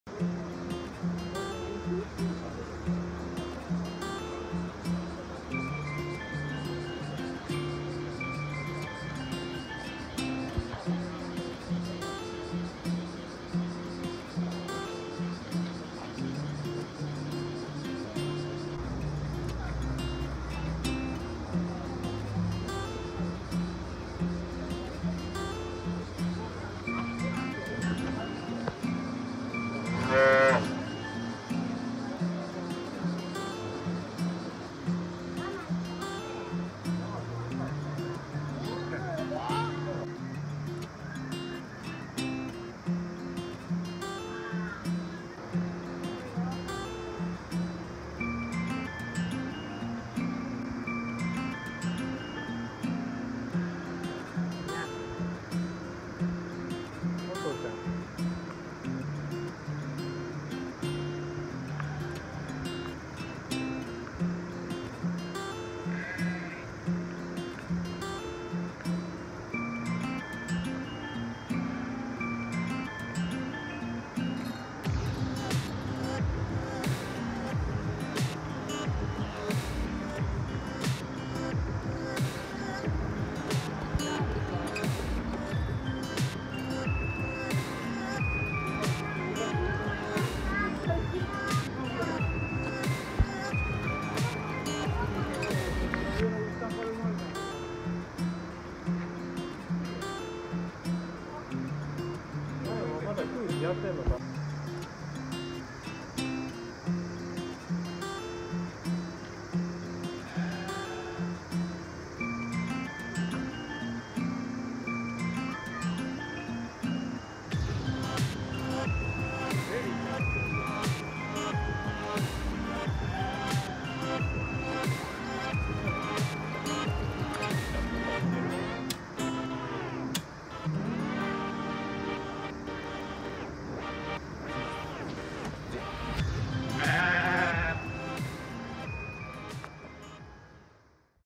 双子の仔牛うたおと【六甲山牧場】生後２週間のちっちゃな仔牛 双子のうたちゃんおとちゃん❤
『六甲山牧場』神戸市灘区
ひまちゃんの双子の仔牛「うたおとちゃん❤」生後２週間の可愛い仔牛ちゃん はじめましてっ♪

動画に出てくる動物たちの詳しい情報はコチラ↓
会いに行ける動物たち
兵庫県神戸市灘区六甲山町中-里山1-1
「六甲山牧場」